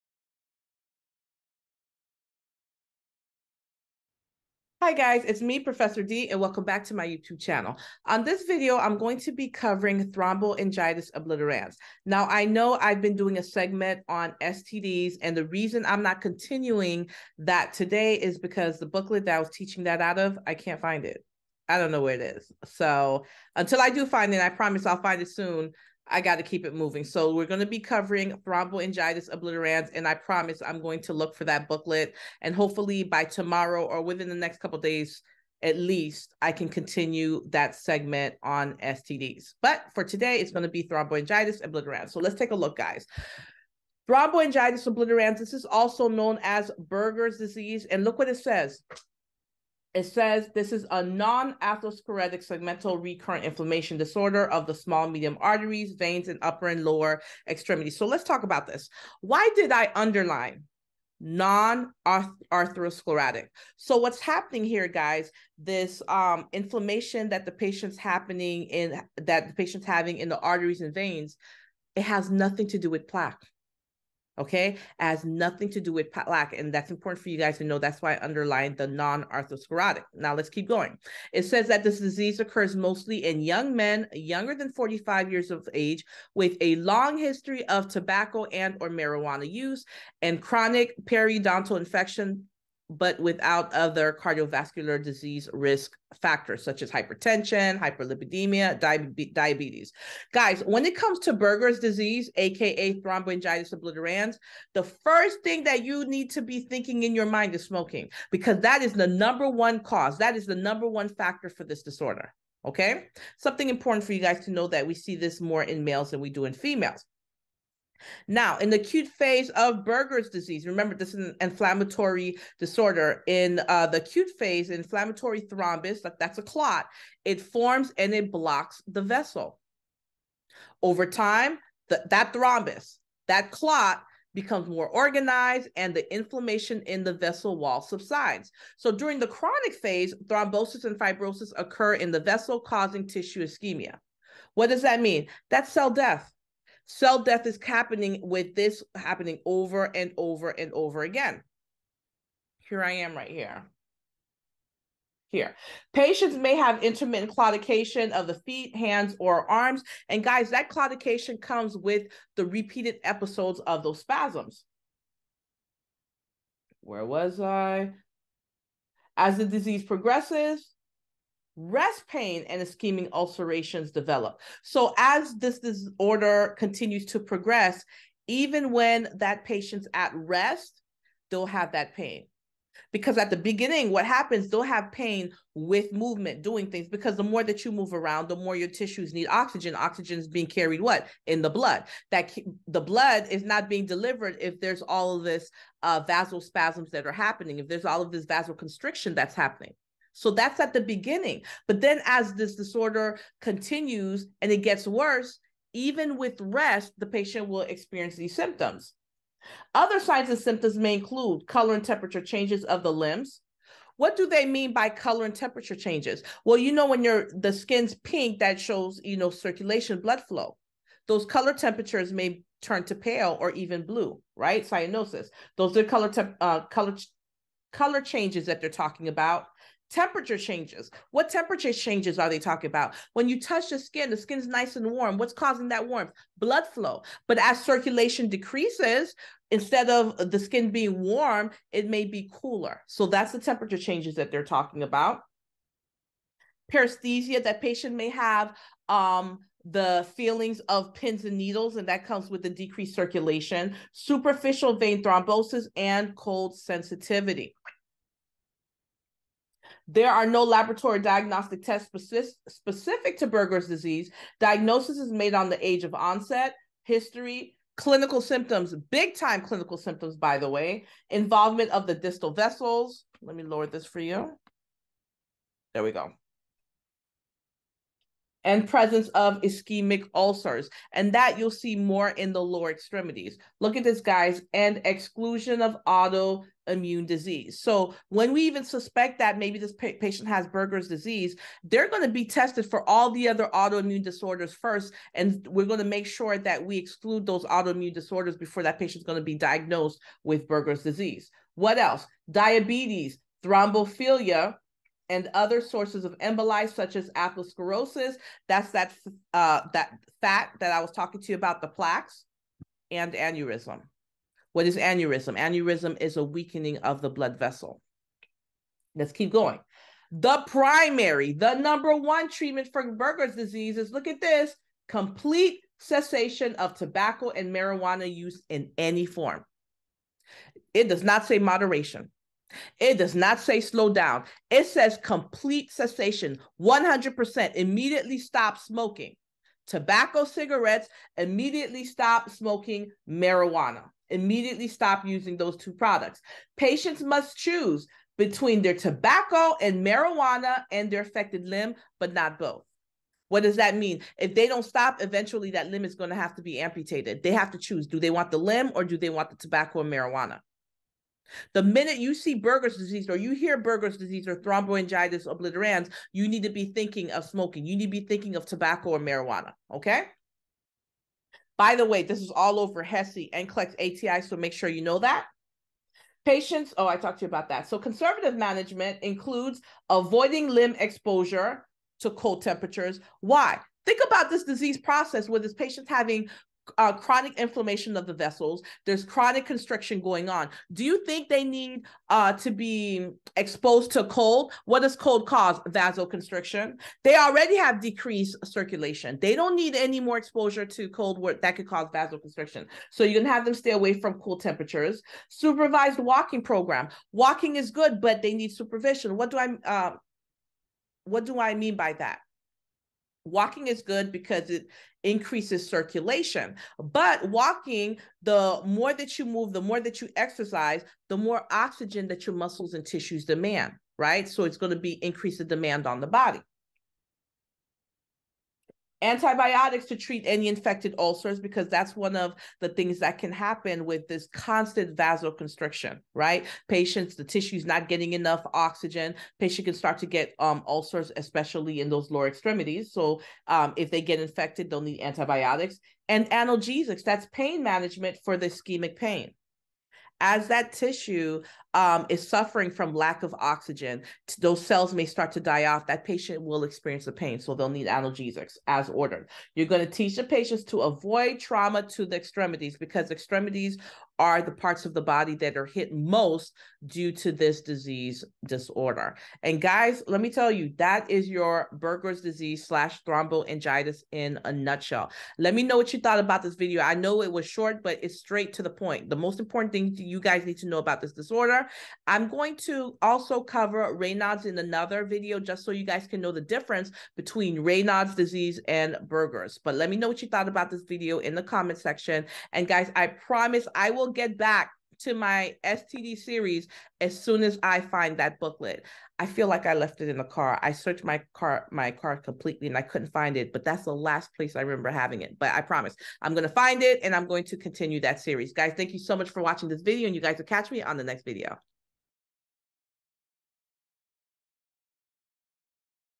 Thromboangiitis Obliterans (Buerger's Disease) in Nursing
In this video, you will learn about Thromboangiitis Obliterans, the clinical manifestations, and nursing interventions for this disorder. This is a great video to add to your nursing studies.

Follow Nexus Nursing on TikTok, Instagram, and Facebook!

Gifts of appreciation welcome!